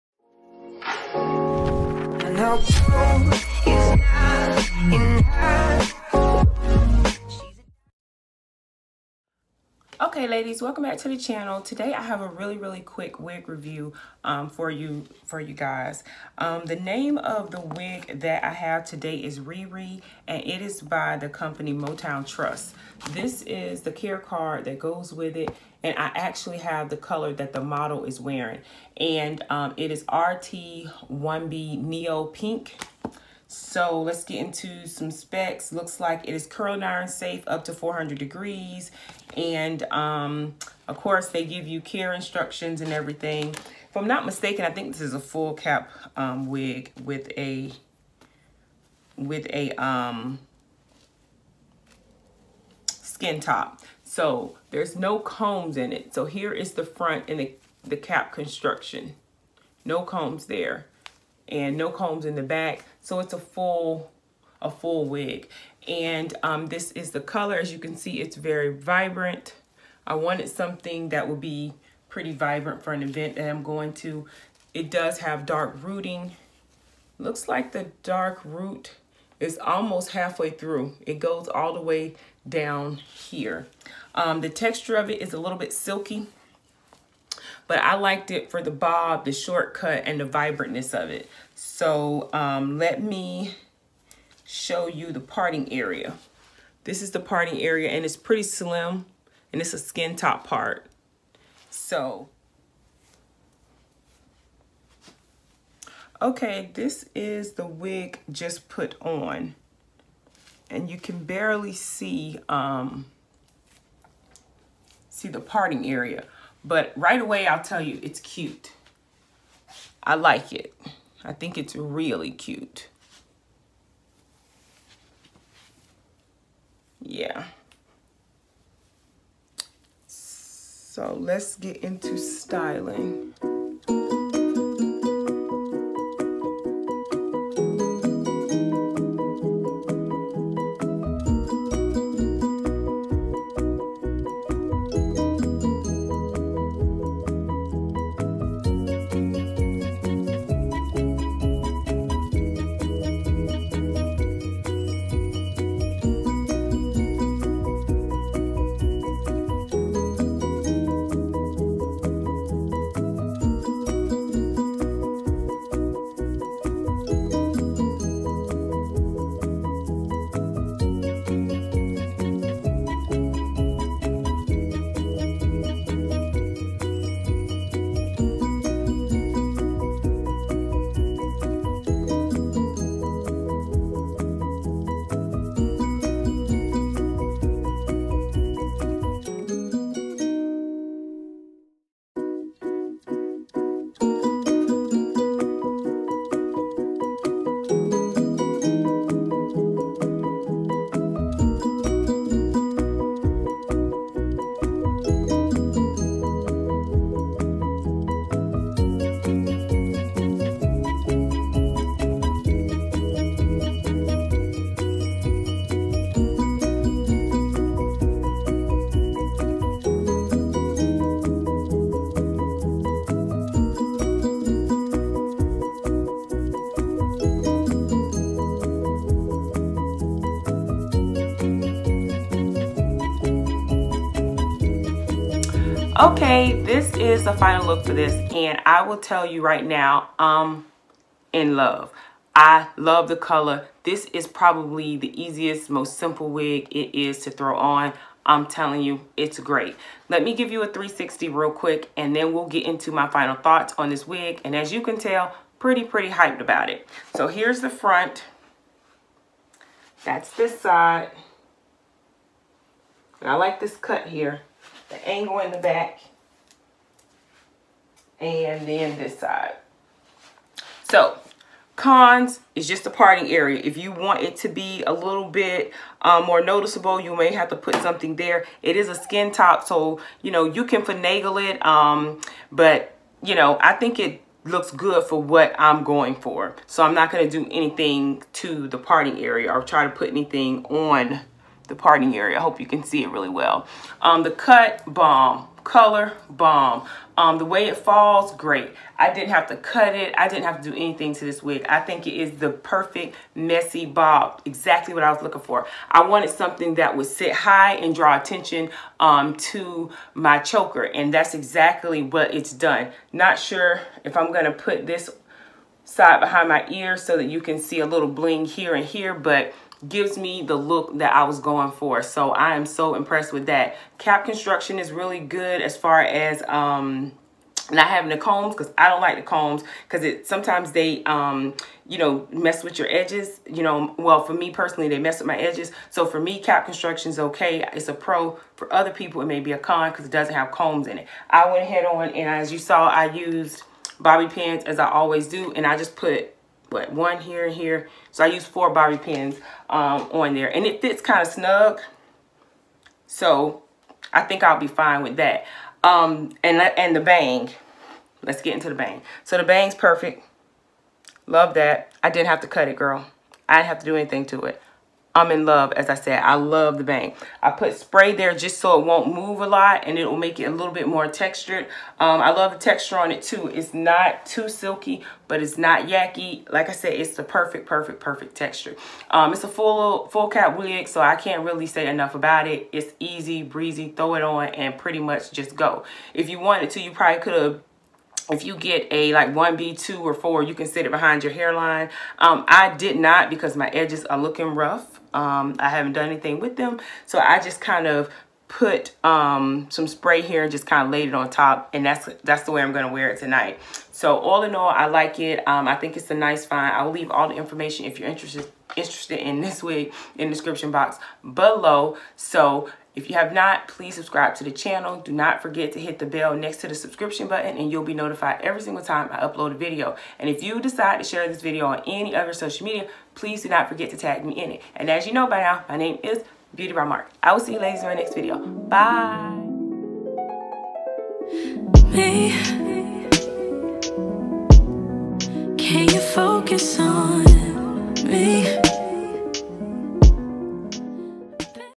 🔥🔥 HOT PINK HAIR 🔥🔥 | MOTOWN TRESS- RIRI | BEAUTIEBYMARK
This video is about MOTOWN TRESS RIRI  WIG in the color RT1B/NEOPINK. This is a full cap wig with a skin top park.